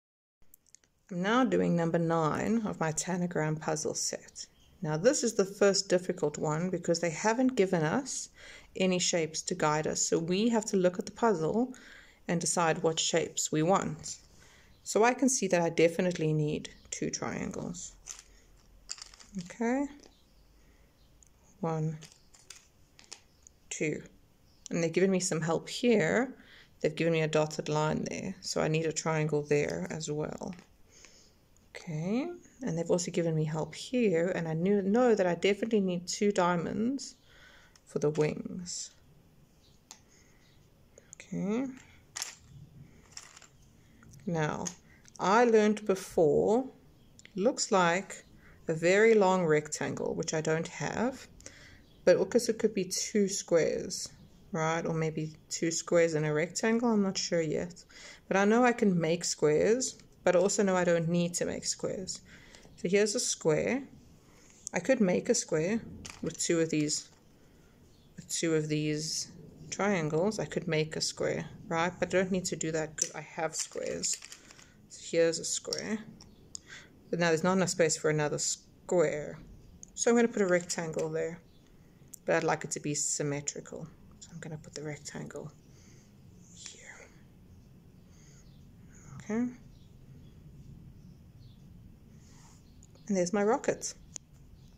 Tanagram rocket ship shape building puzzle for kids (card 9)
Tanagram puzzles are a great tool for developing early problem solving skills. It develops STEM and coding language and thinking patterns which young children will need later in life. It also develops logic and spatial thinking skills. Learn shapes, colours and positions and how 2 shapes can make a new shape.
The early puzzles in this set are suitable for very young children aged 2 up and the later ones would be suitable for children up to 8 or so.